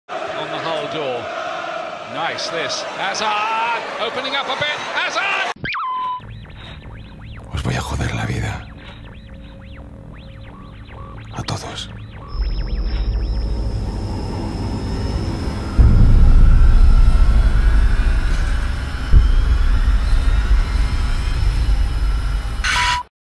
Tráiler Círculos
Tráiler de la novela Círculos, del guionista Manuel Ríos.